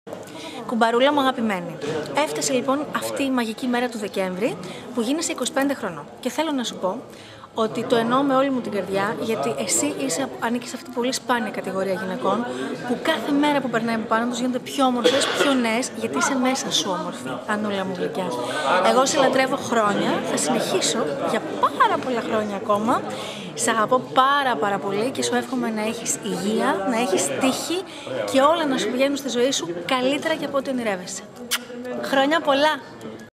Natalia Germanou Wishes Anna Vissi Happy Birthday [fannatics.gr]
Natalia Germanou Wishes Anna Vissi Happy Birthday

Anna has her birthday on December 20th.